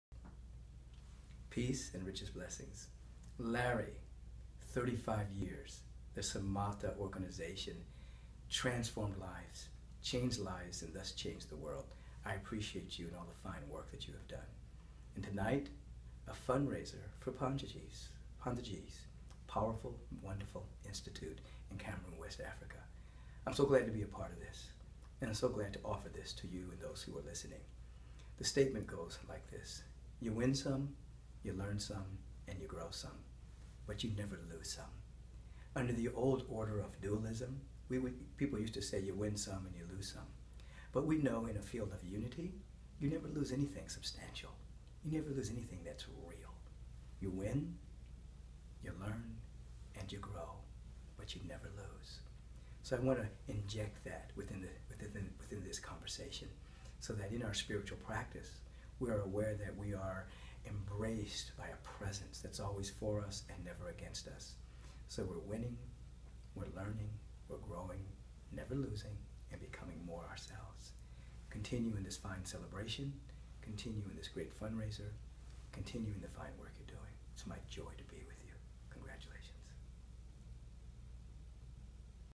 Rev. Michael Beckwith's Gift That Keeps On Giving: a 35th Anniversary Celebration by Samata Yoga
Wisdom: "You win some, you learn some, and you grow some, but you never lose some." We are embraced by a presence that is always for us and never against us.

In 1986, Michael Bernard Beckwith founded the Agape International Spiritual Center, a trans-denominational community of thousands of local members and global live streamers. He is a sought after meditation teacher, conference speaker, and seminar leader on the Life Visioning Process, which he originated.

Click this link if you would like to learn more or donate